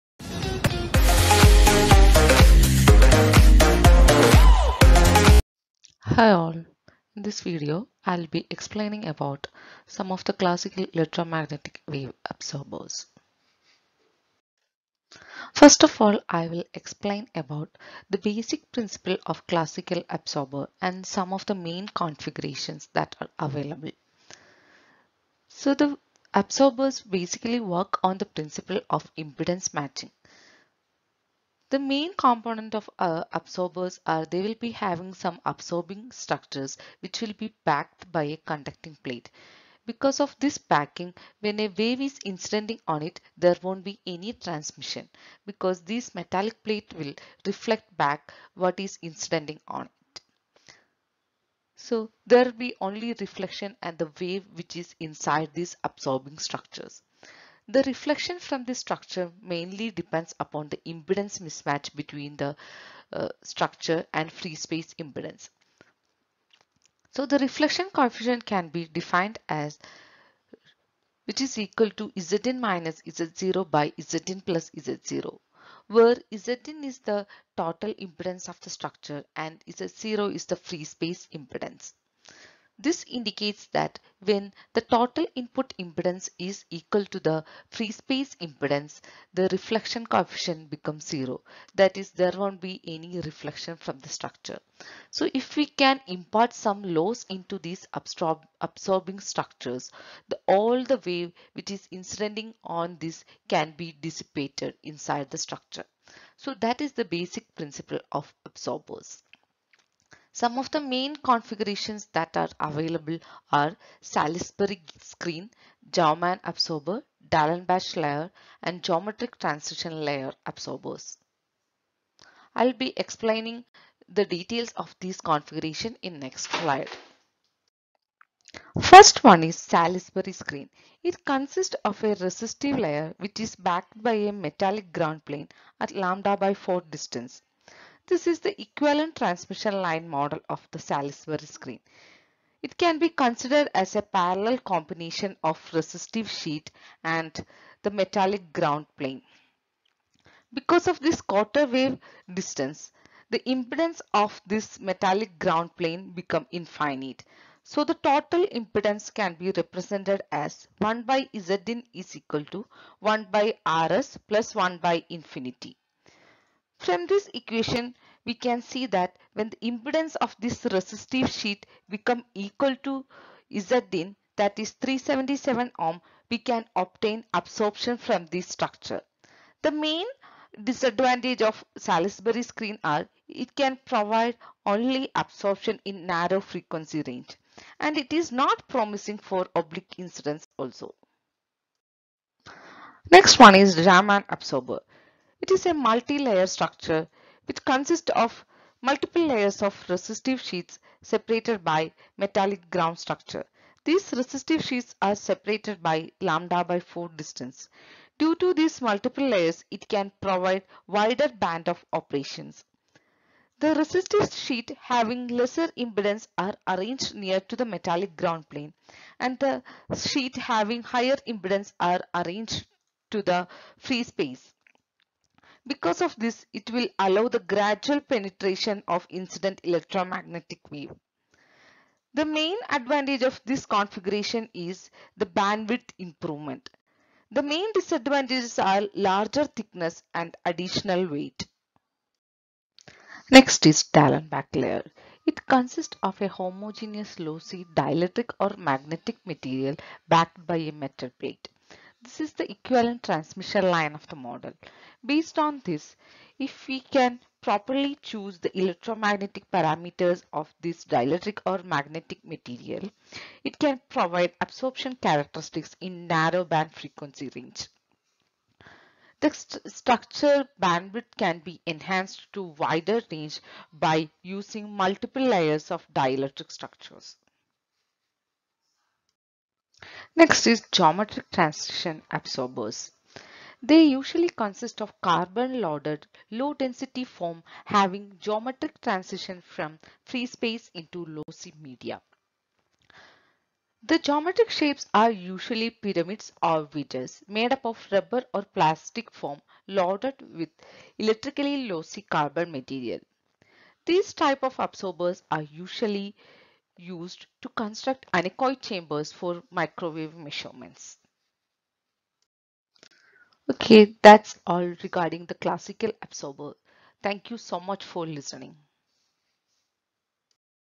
Classical Electromagnetic Wave Absorbers
This video describes about the classical electromagnetic absorbers like Salisbury screen, Jaumann absorbers etc..